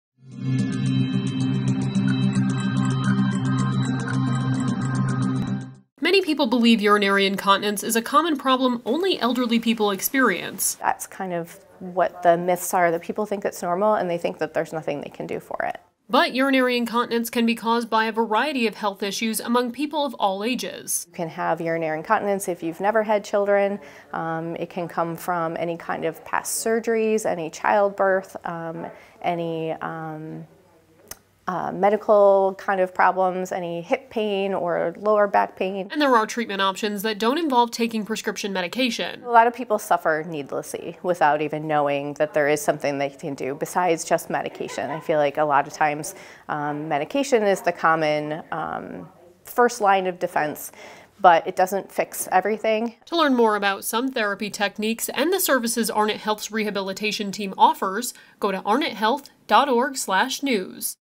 Arnot Health Minute - Urinary Incontinence Therapy
Arnot Health physical therapist Jennifer Gehl, a certified pelvic floor specialist, talks about the causes and treatments for urinary incontinence.